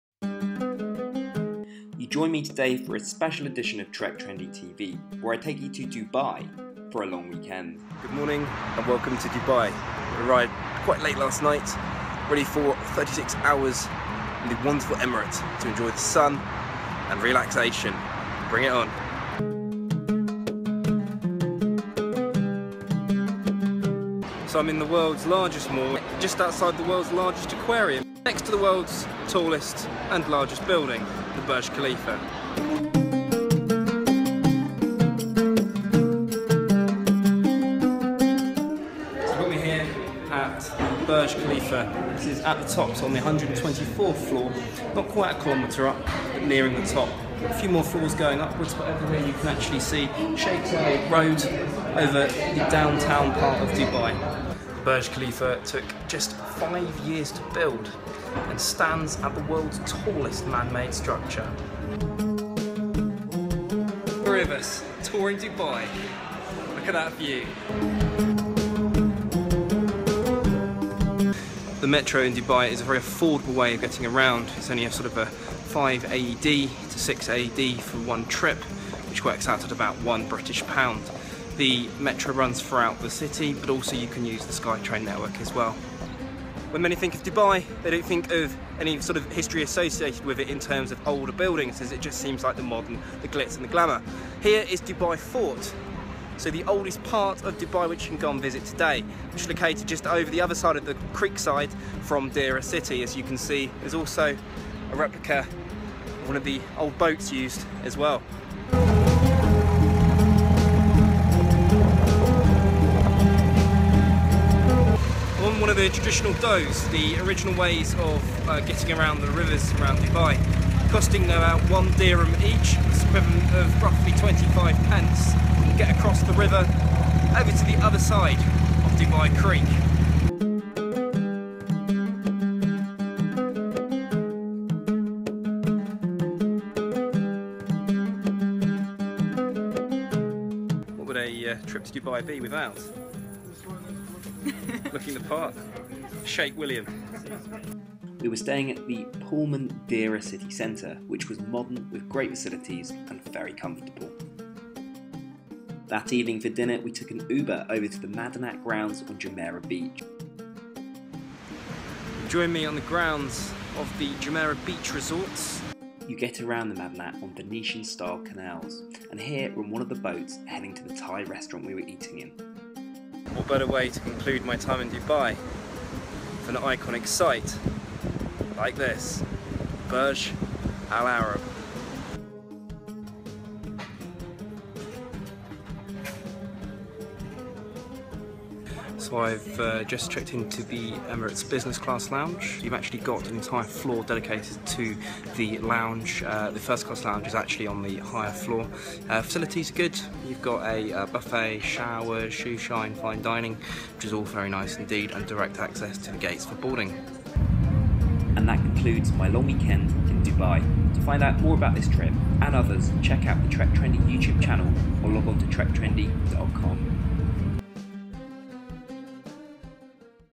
TREKTRENDY DOES DUBAI SHEIKH SYLE! - SE of 12
Along my journey to do 12 countries in 12 months, I spend a long weekend in Dubai. Seeing as I'd already visited the UAE in February 2015, I thought i'd put together this special edition of TrekTrendy TV. Watch where I climb to the 124th floor of the Burj Khalifa, explore the Dubai mall, sail along the venetian canals of the Madinat and have a selfie in a traditional arabic Thawb!